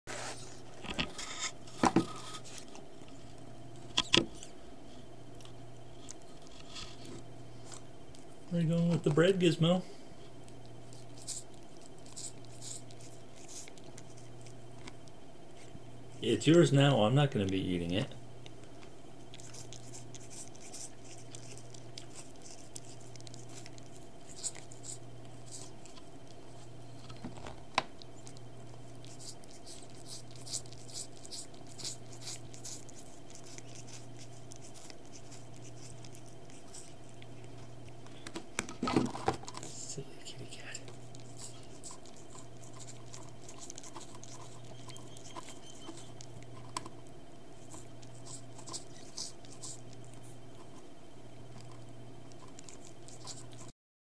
Gizmo Likes Bread, Too
Scout was eating bread last week but now Gizmo got to the loaf first.  Gizmo has been featured here before.  He's our geriatric, diabetic, grumpy old man of a cat that likes rolling around on the lawn.  He's got a hold of the trencher bread recipe I'm trying out for the first time.  Verdict on the bread: it's good but I need more practice making it.  Verdict on the cat: he's a greedy little fuzzbutt.